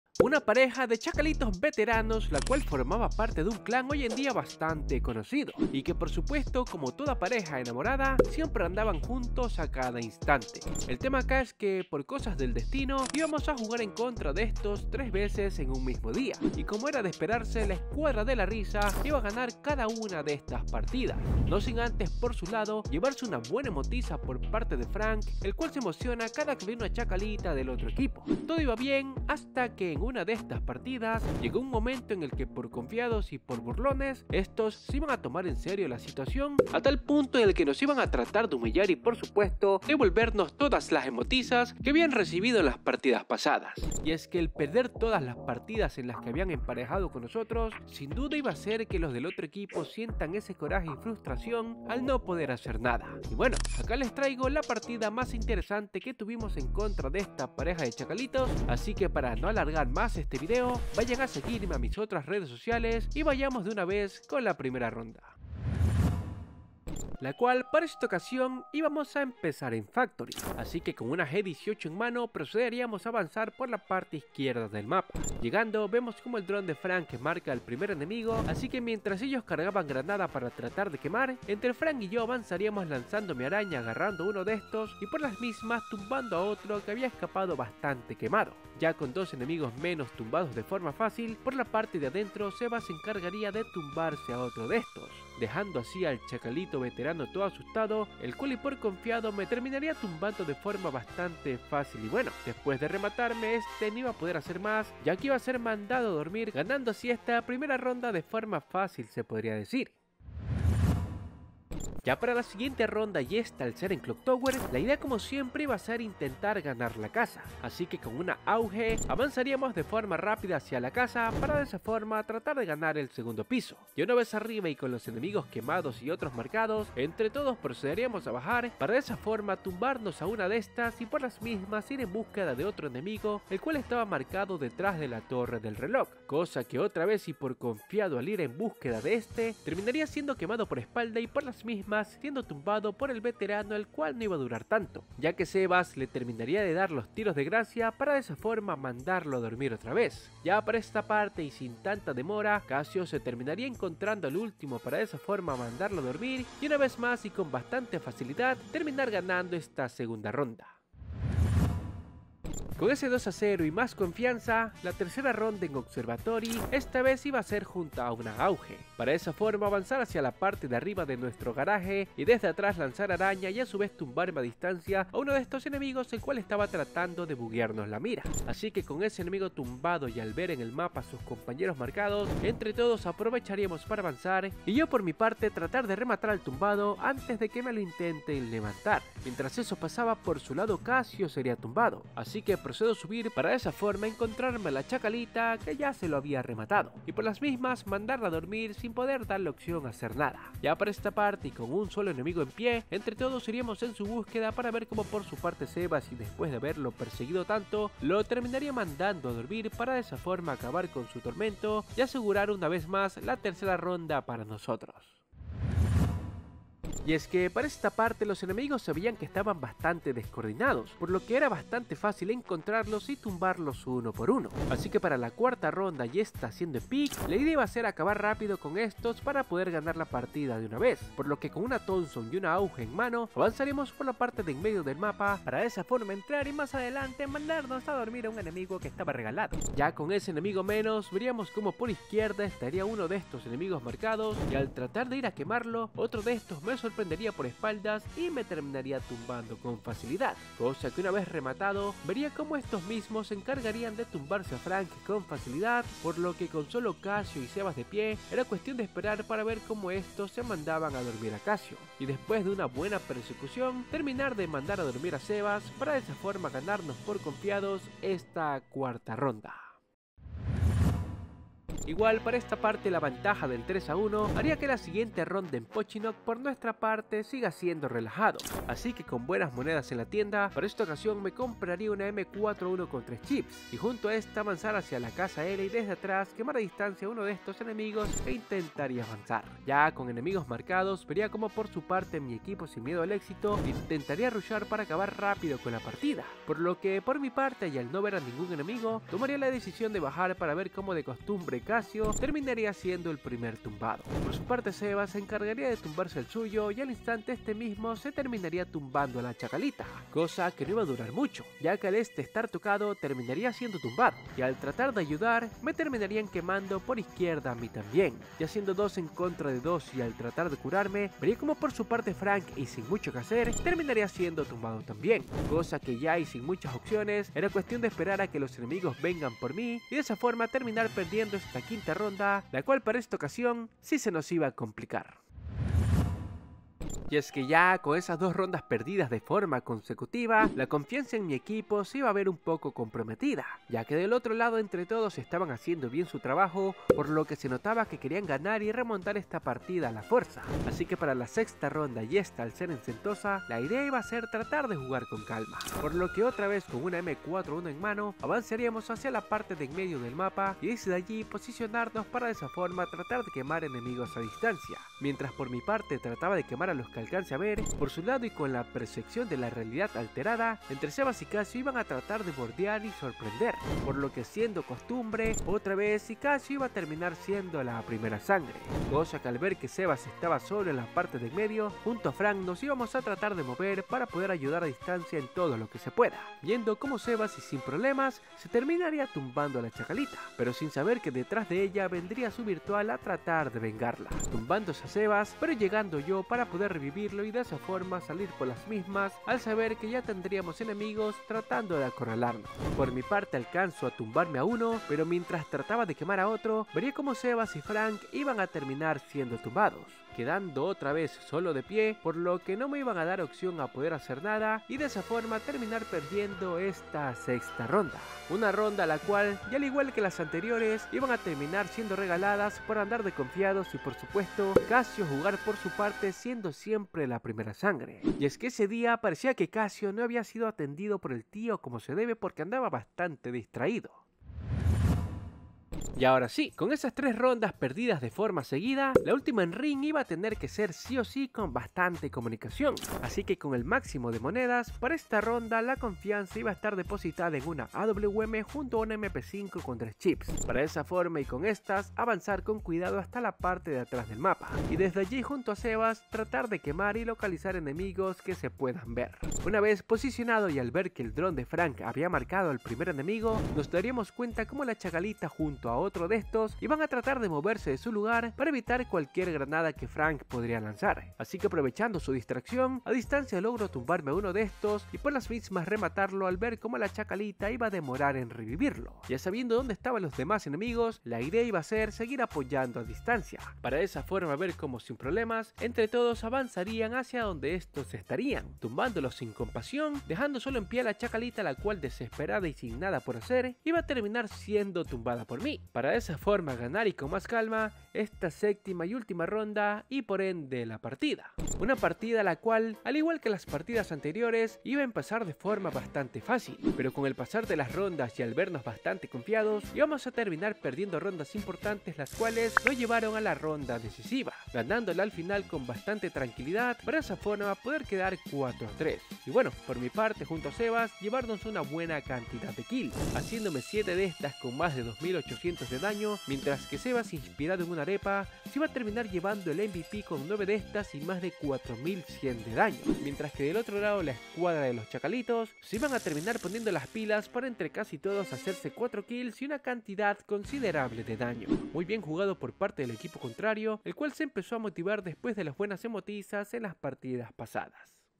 ESTOS VIRTUALITOS NOS EMPAREJARON 3 VECES en busca de su REVANCHA 😳
Una parejita de virtualitos nos emparejo 3 veces en busqueda de su revancha